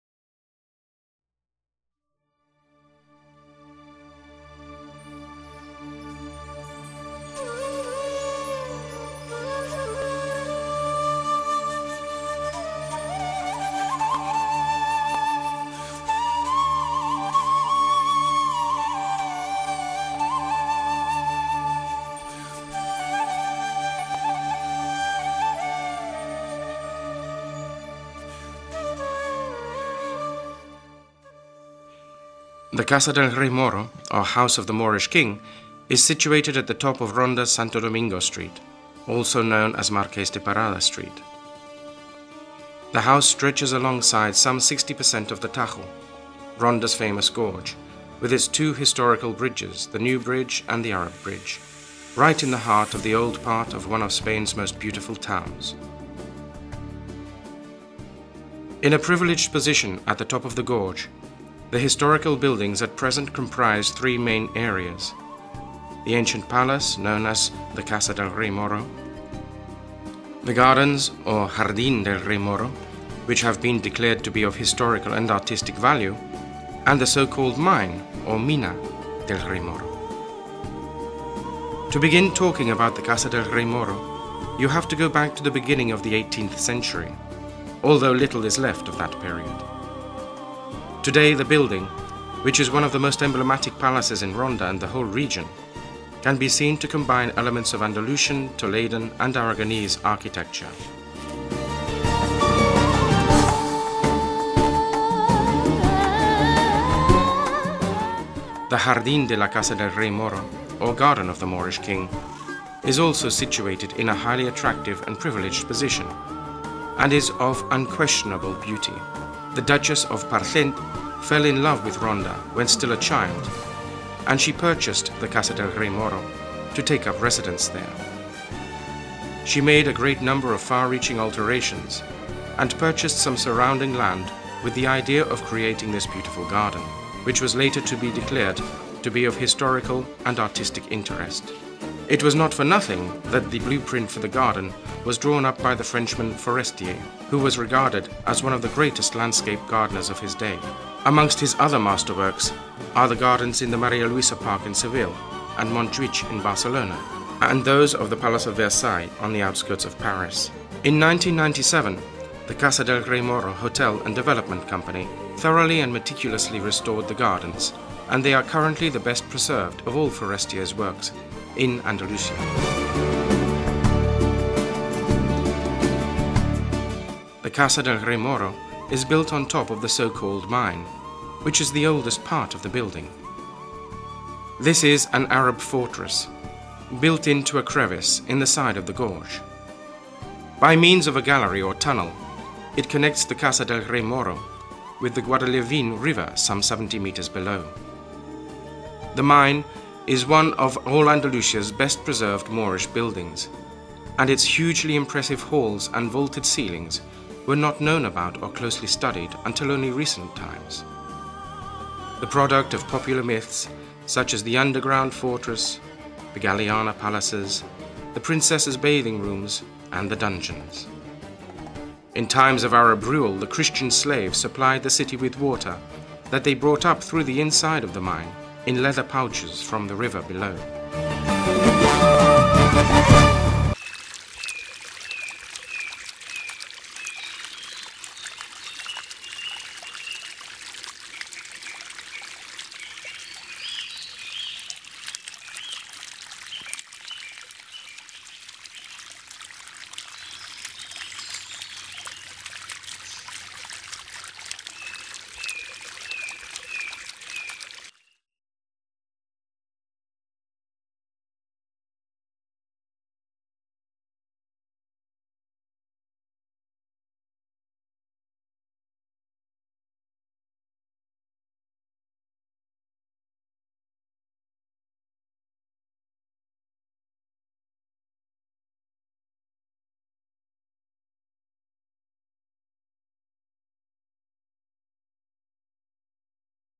Palace of the Moorish King, Ronda (Málaga) - Michelle Obama many thanks for your vist
Palace of the Moorish King, Ronda (Malaga) with it´s garden of Forestier and the arabic "Mine" from XIII. century.